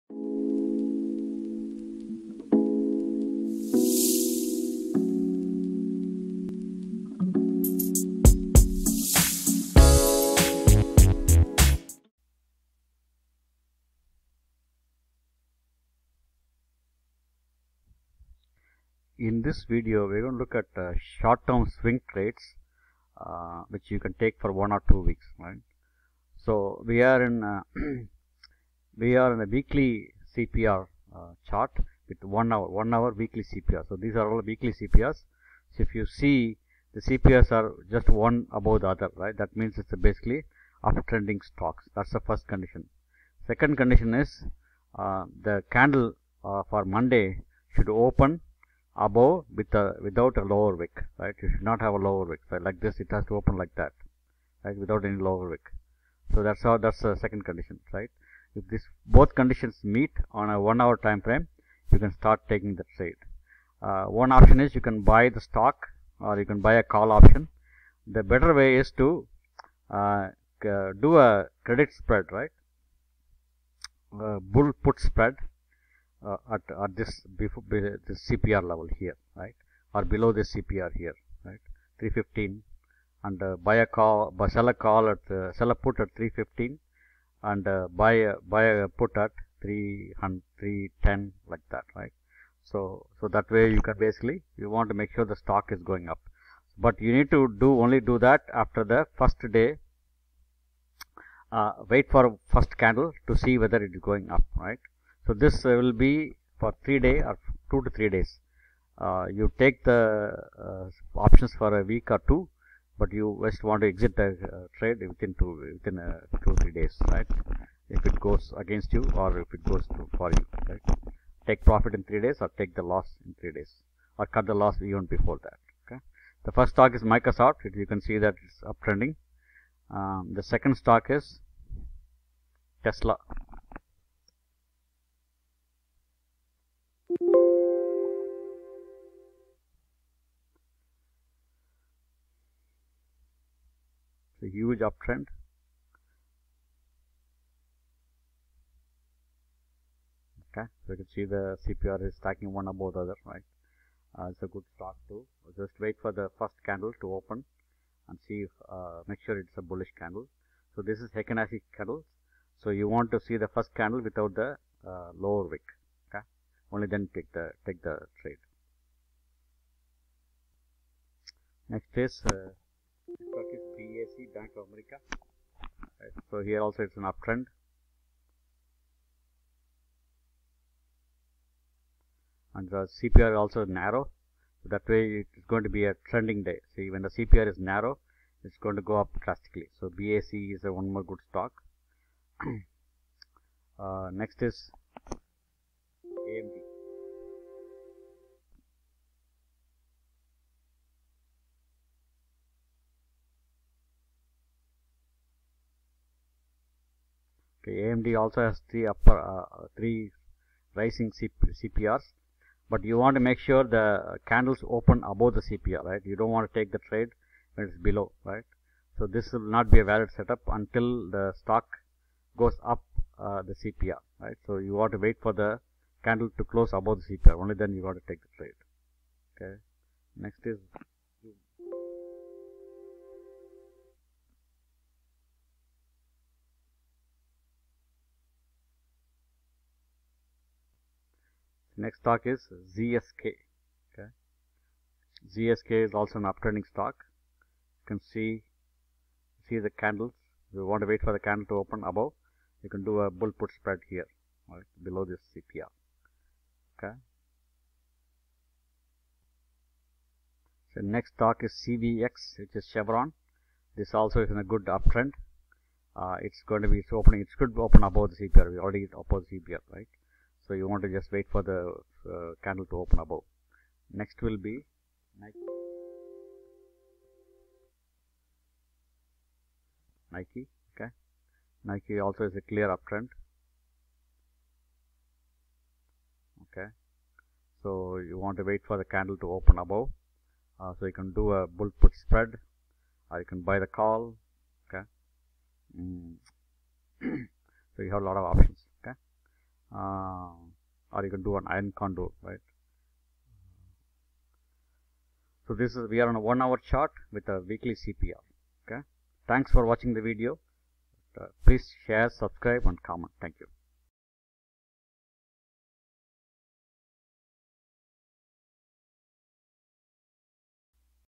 Swing Trading base on Ascending Weekly CPR for November 1st -5th 2001
In this video, we are looking at stocks for Swing Trading. These stocks are based on scans for up-trending weekly CPR. We will use the hourly chart with a Heikinasi candle. Make sure the candle is above the Weekly CPR without a lower wick. Watch the char one hour before the close of the session and take the trade.

Exit the trade when the candle turns red or closed below the Weekly CPR. Watch these stocks daily and take action to buy or sell. You can buy the stock, sell bull put credit spreads, and also buy just the call. This will depend on your funds.

Following are the stocks which have three consecutive CPRs up
Weekly CPR Up
MSFT
TSLA
BAC
ZOM
CVX
NKE
AMD
GSK